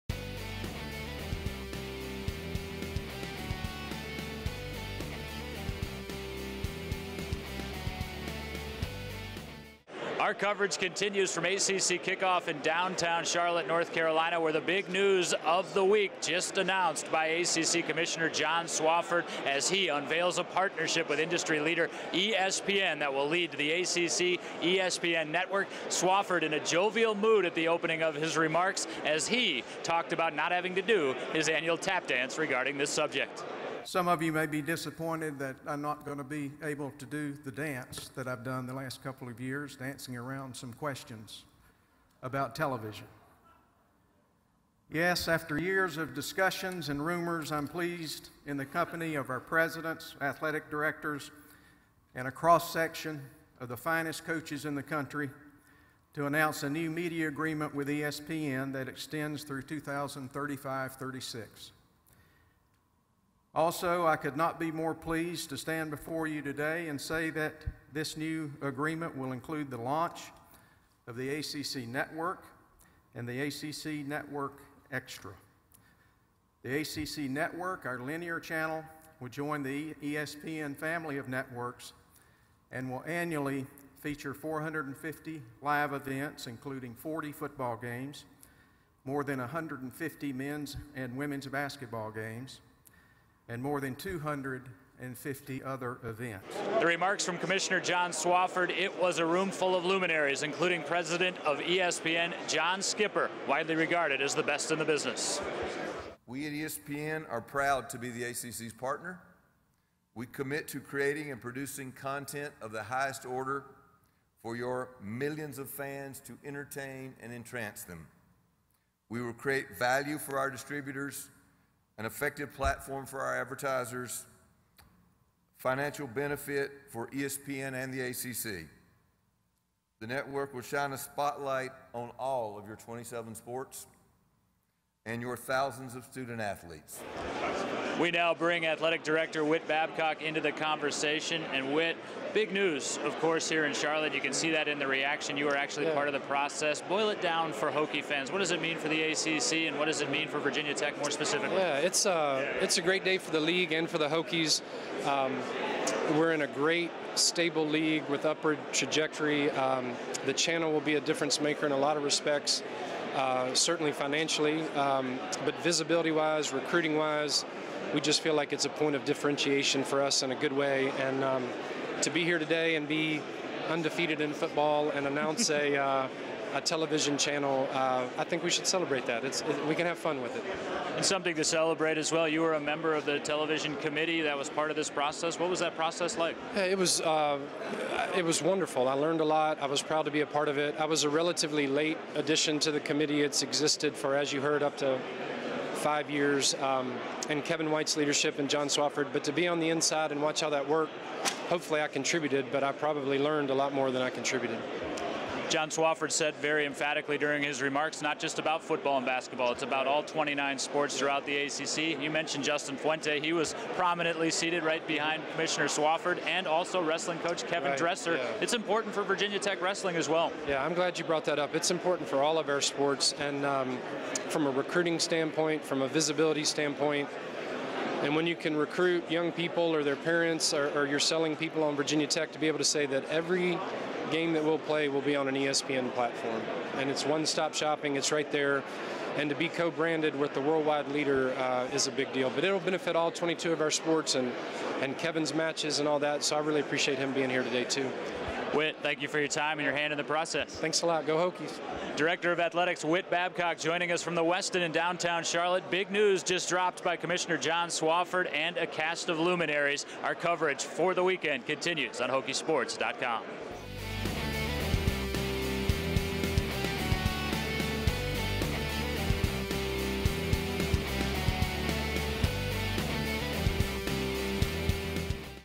ACC Kickoff - ACC ESPN Network Announcement
Jon Laaser catches up with VT Director of Athletics Whit Babcock for his thoughts about the ACC's partnership with ESPN.